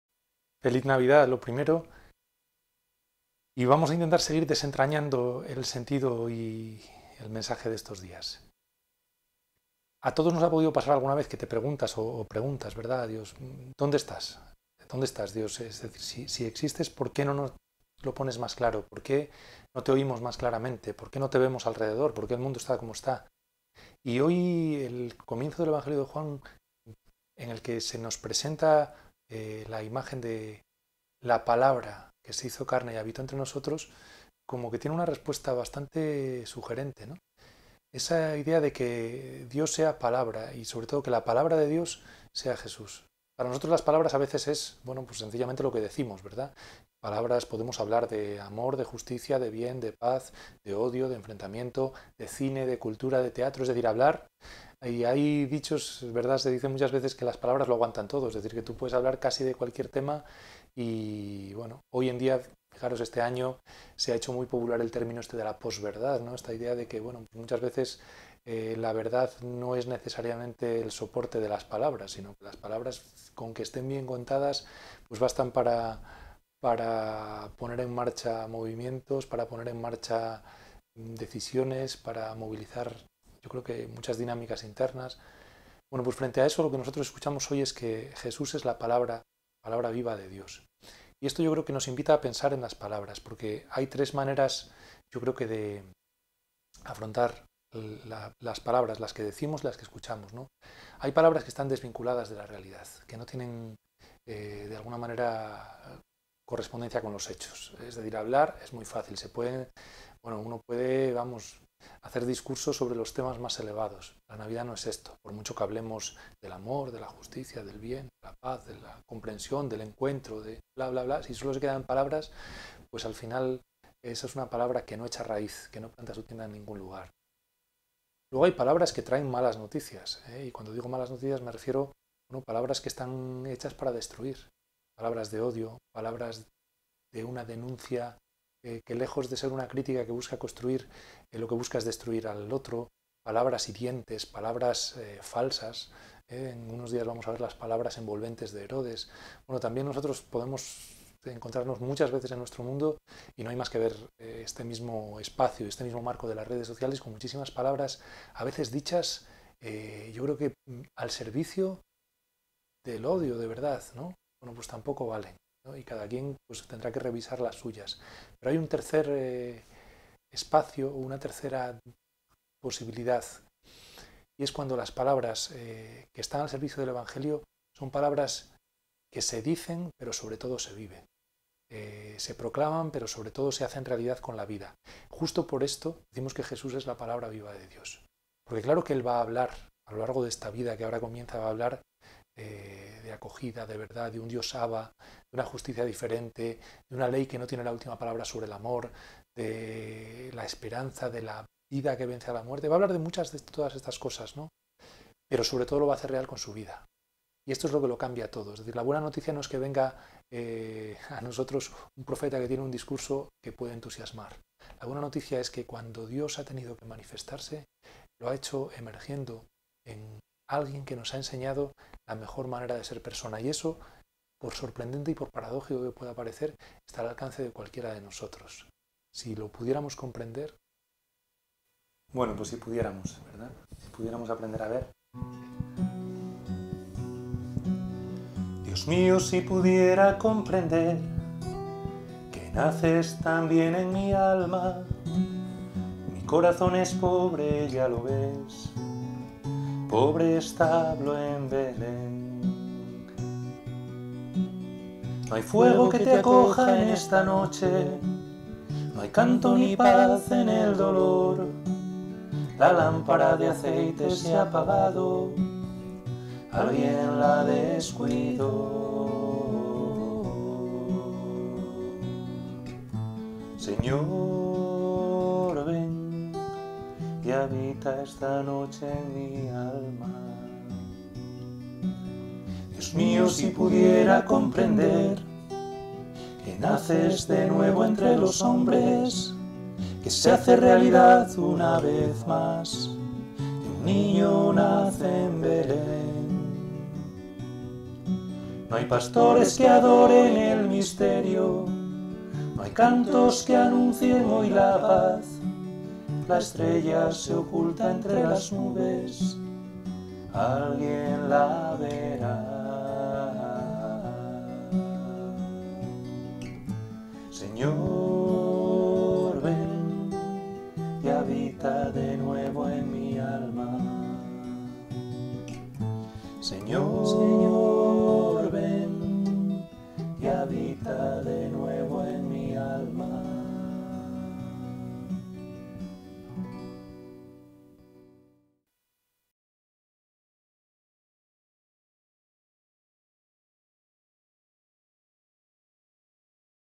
Desentrañar la Navidad: La Palabra, entre nosotros
Las palabras, tan poderosas, tan certeras, tan capaces de lo mejor -aunque también de lo peor-. Palabras que, si acompañan a las obras, son auténticas, pero que si se quedan en pura fachada, son humo. Palabras con las que podemos amar o destruir. Palabras como la Palabra que evocamos en Navidad...